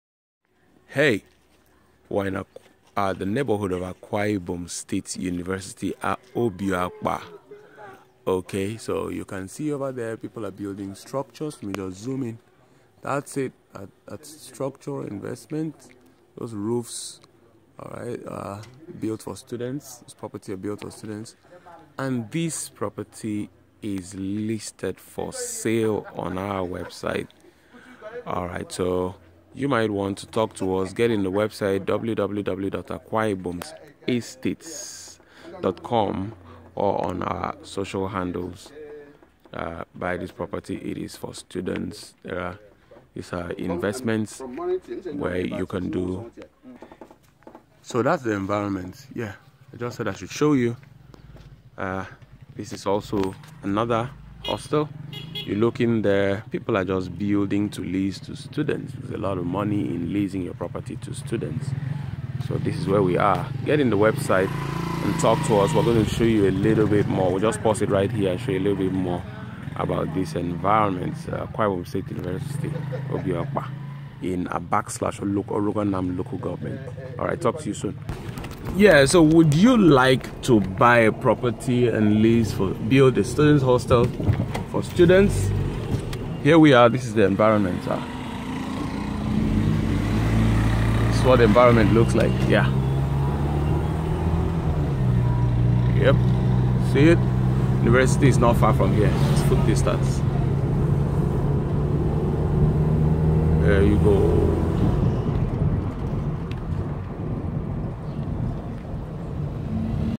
Build A Hostel in Akwa Ibom! Buy this property near Akwa Ibom State University Obio Akpa Campus
Type of property - Uncompleted building near Akwa Ibom State University, Obio Akpa at Oruk Anam LGA
Size of land - At least 600 sqm (different plots joined)
Neighbourhood - Akwa Ibom State University, Obio Akpa campus (Abak/Oruk Anam)
Access - Tarred road, within 2 minutes stroll

We have other property for you to check out. See a video of Uyo's very cheap property, N950,000 per plot.